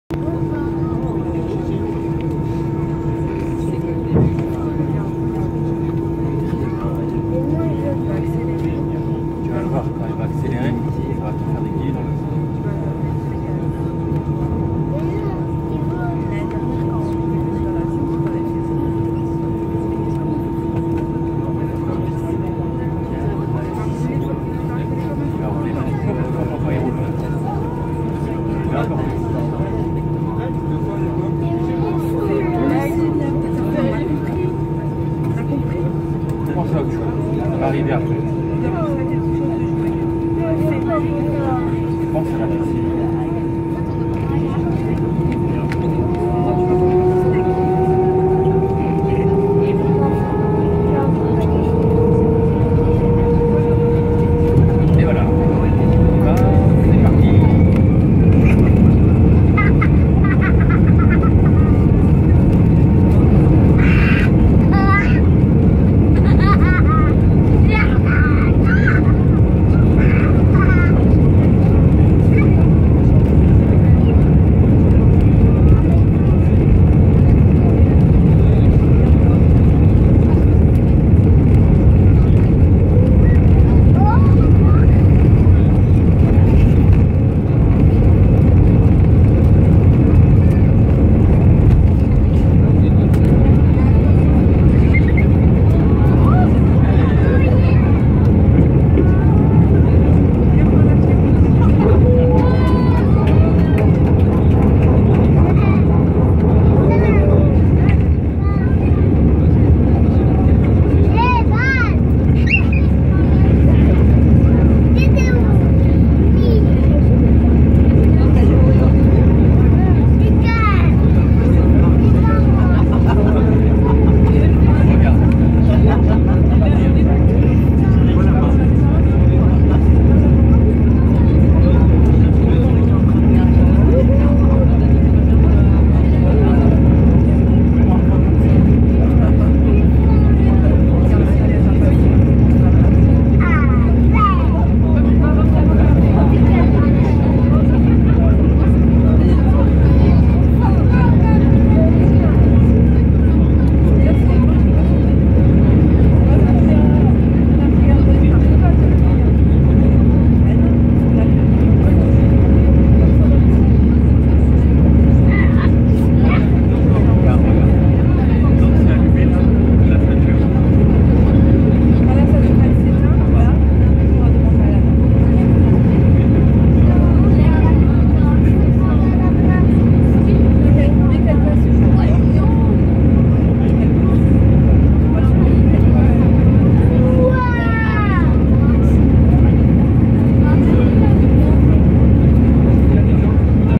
Brussels Airlines take-off Brussels (2019)
Brussels Airlines
Take-off Brussels Airport
Destination Djerba
Airbus A320